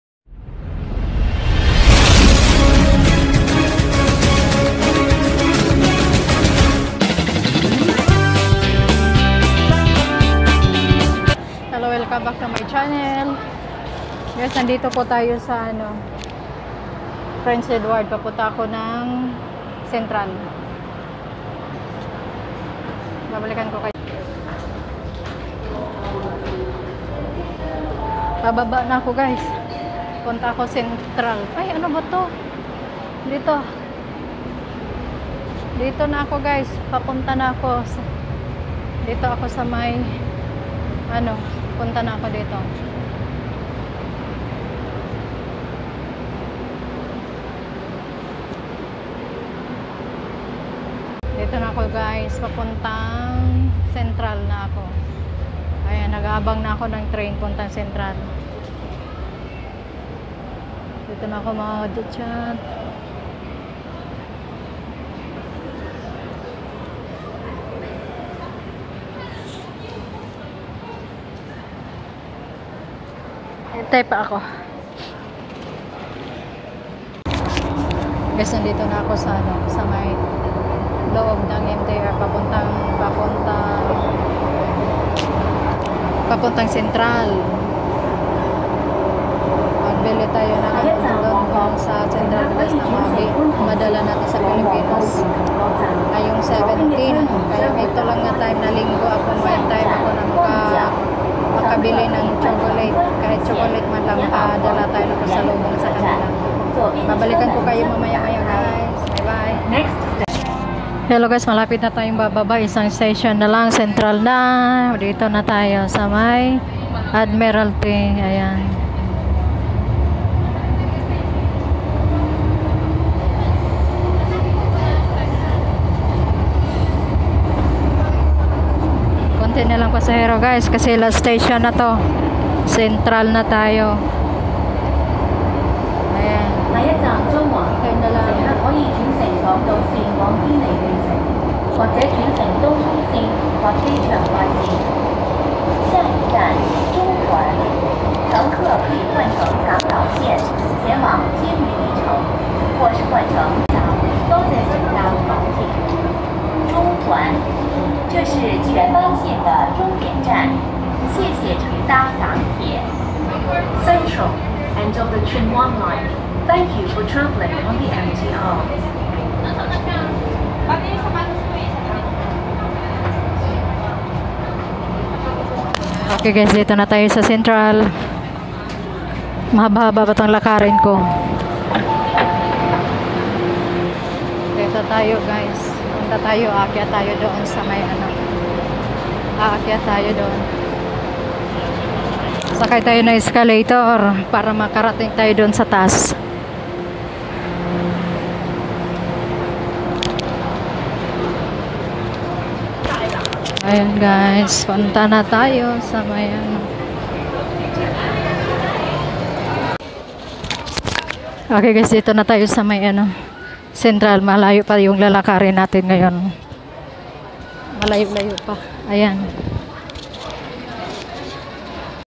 Bumili ng Pasabong || Mura ang chocolate sa Price Mark || 8/16/2019
Bumili ng chocolate sa Price Mark mura pasalubong.